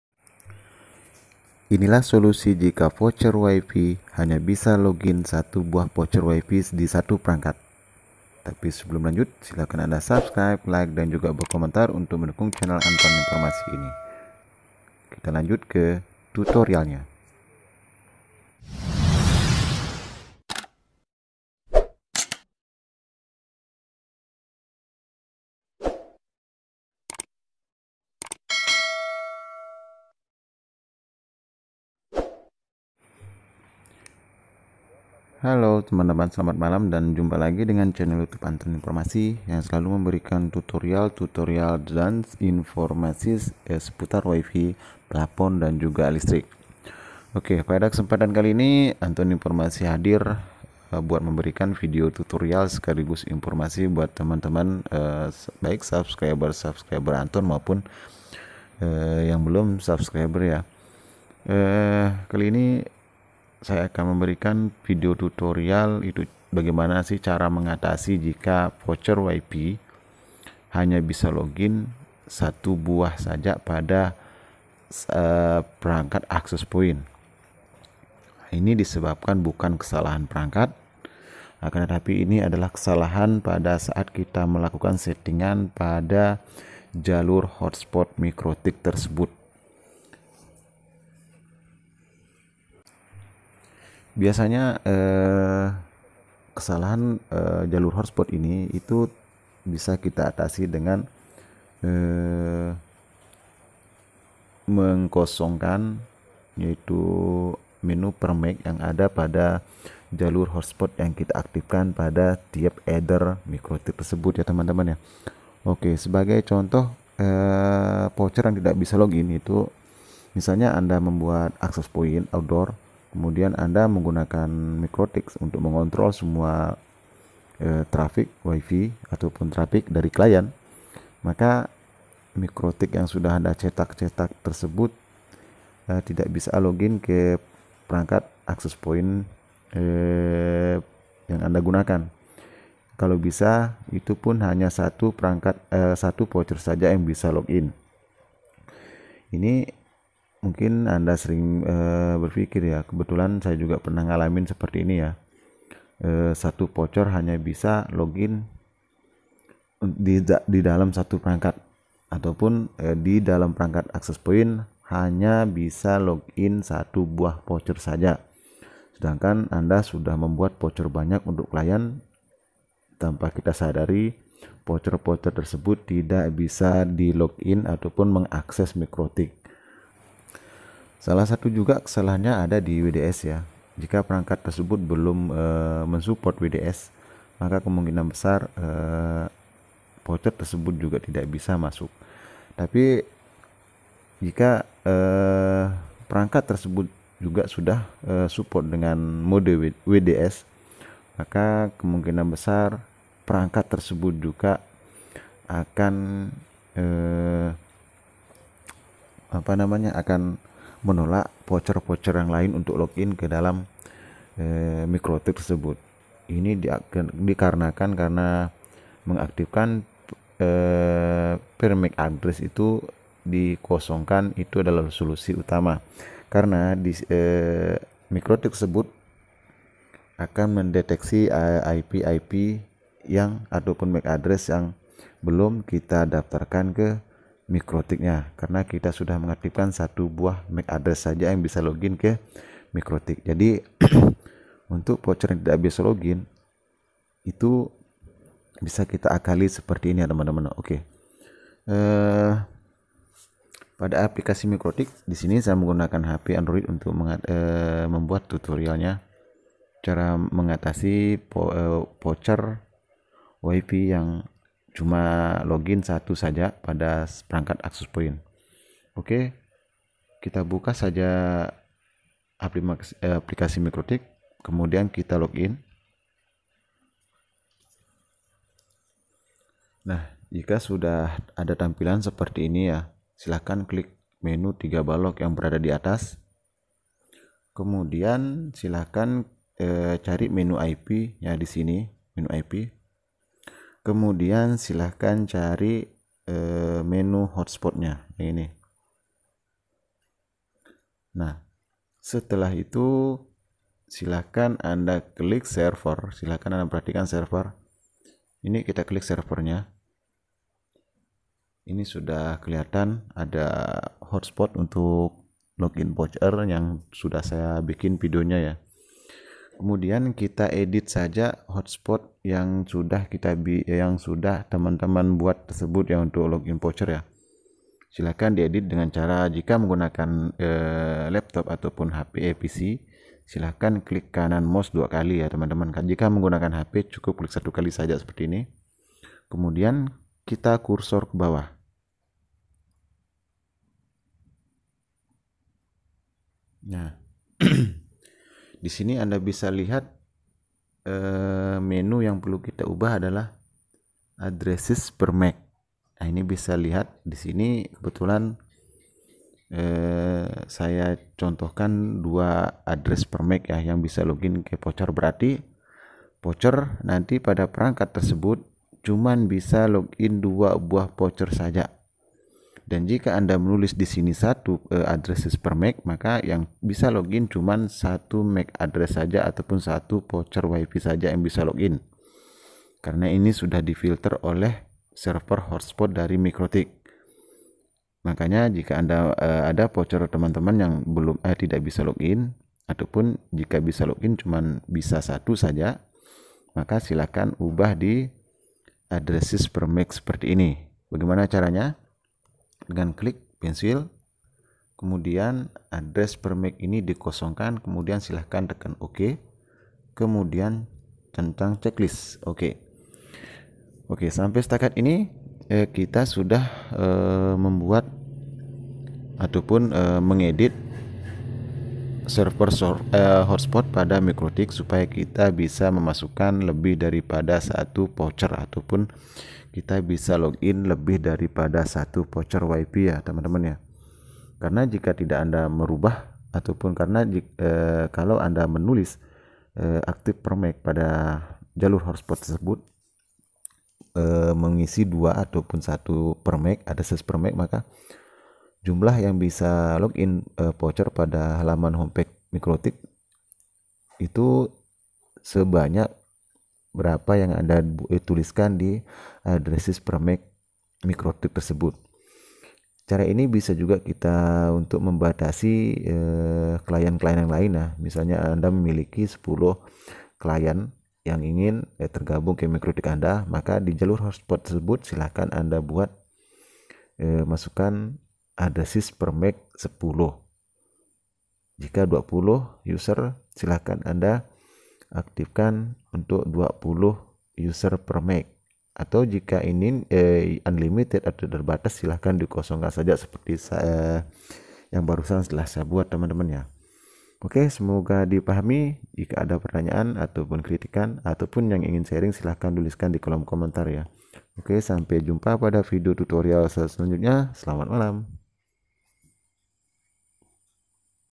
SOLUSI JIKA VOUCHER WIFI HANYA BISA LOG IN 1 USER SAJA
solusi jika voucher  wifi hanya bisa log in 1 voucher saja.
Kembali channel anton informasi memberikan video tutorial dan informasi  dan selalu memberikan solusi seputar wifi.

seperti judul dari video saya kali ini,yaitu solusi jika voucher wifi yang kita buat banyak hanya satu yang bisa log in.

cara mengatasinya silahkan tonton videonya hingga selesai

semoga berkah,amiiin